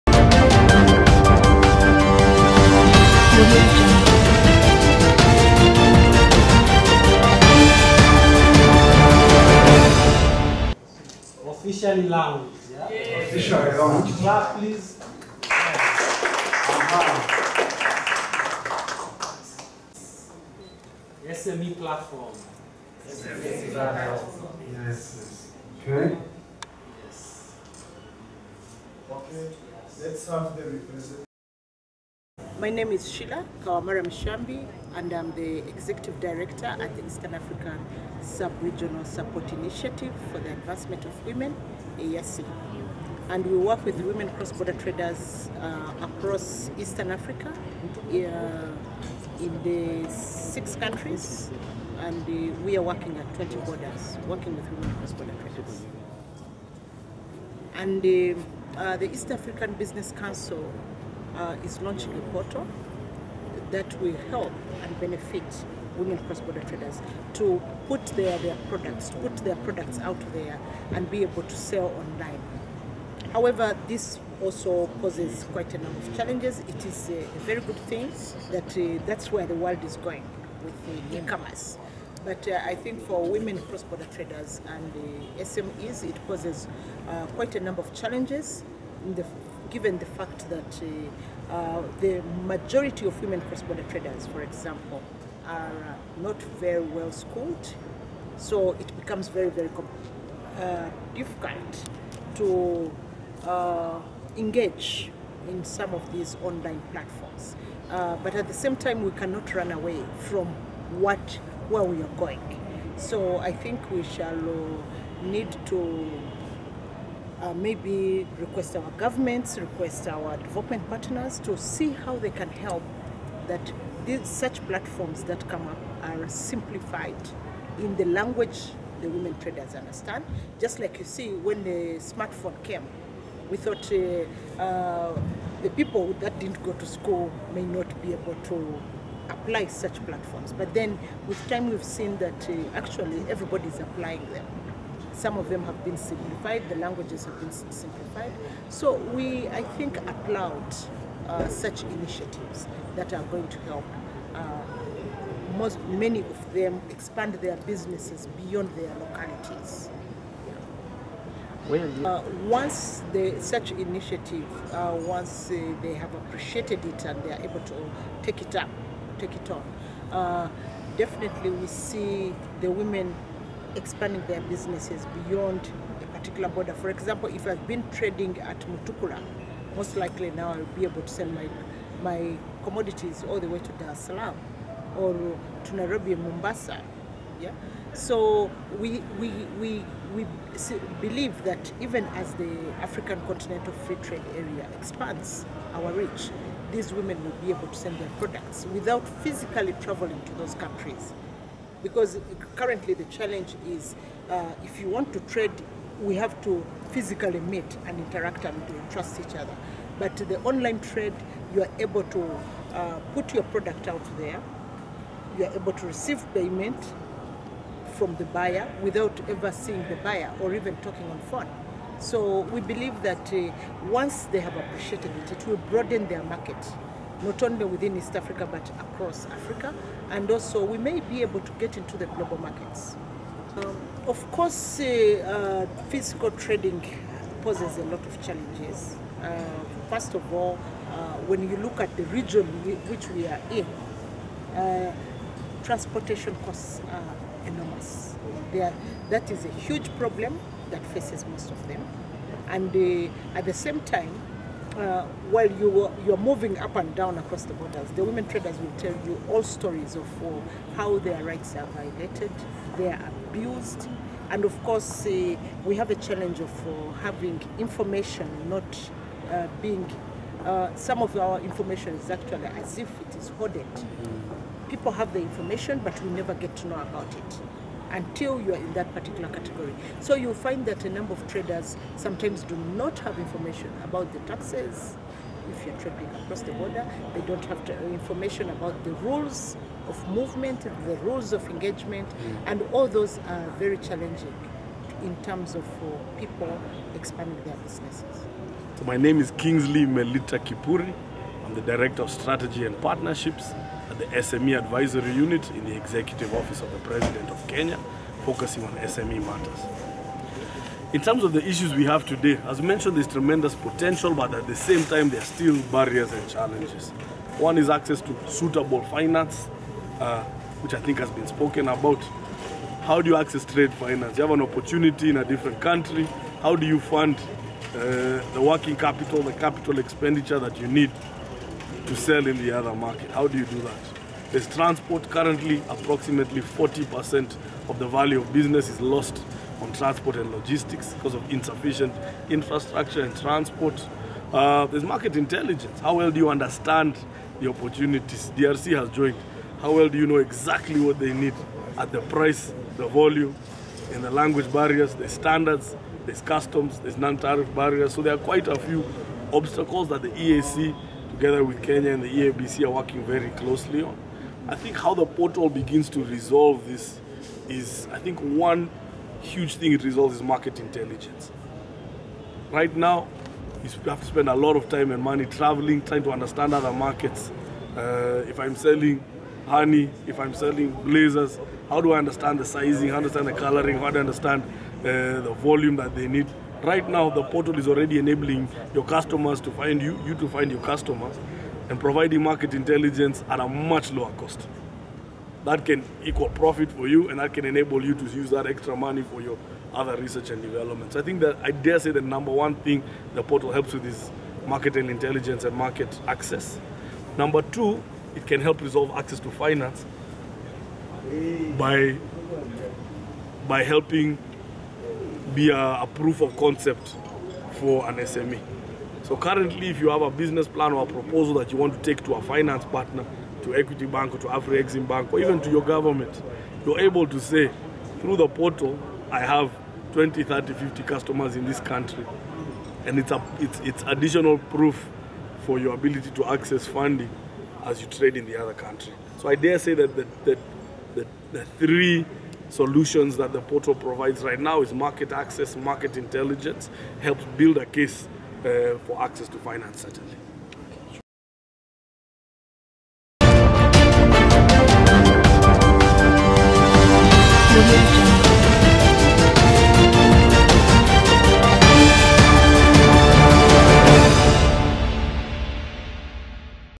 East African SMEs platform launched to enable SMEs trade confidently across E. Africa region& beyond
East African small and medium enterprises SMEs on a recovery path from the devastating impacts of Covid-19 impacts.
East African SMEs platform launched to enable small and medium enterprises trade confidently across E.A region & beyond
This will cut down the transaction cost of trading under the African Continental Free Trading Area (AfCFTA) with a huge market of 1.2billion consumers.@workerstvafrica8038